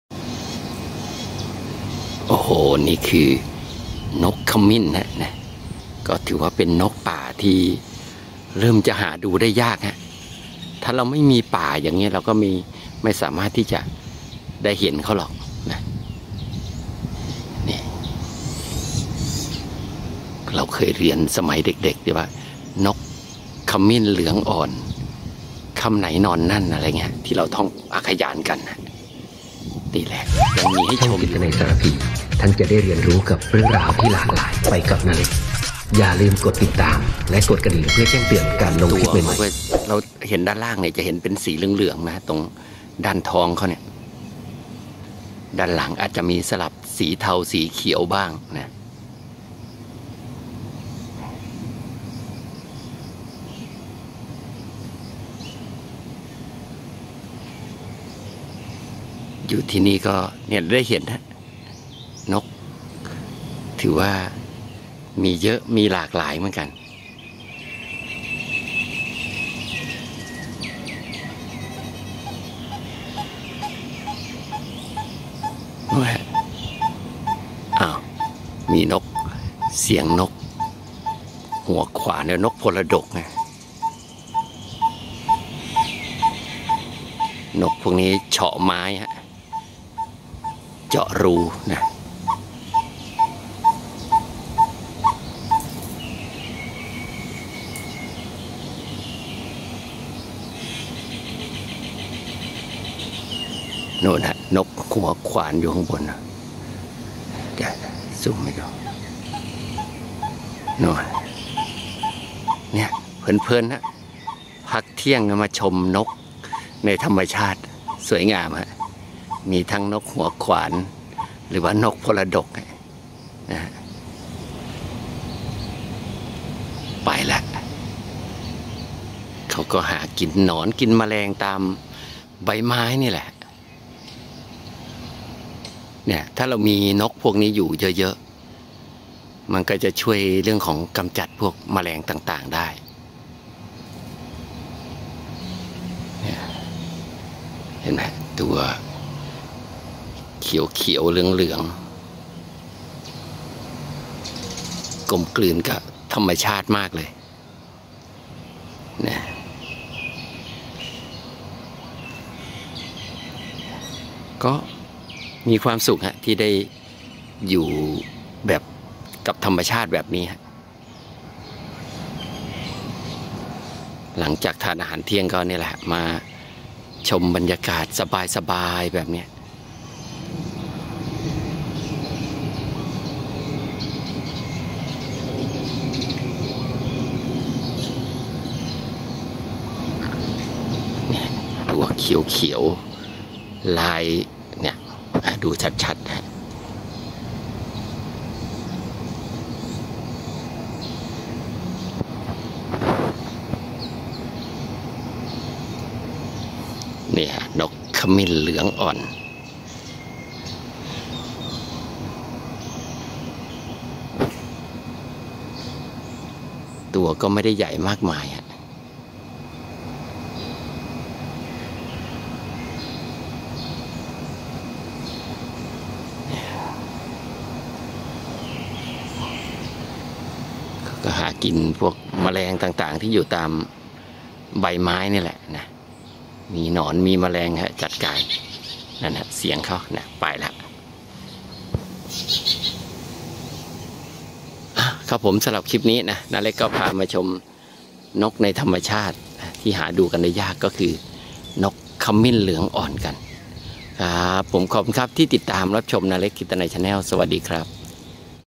ชมนกกับน้าเล็ก#: นกขมิ้นเหลืองอ่อน พักกลางวันเดินชมนกในสวนรพ.สิริกิติ์
# ชมนกกับน้าเล็ก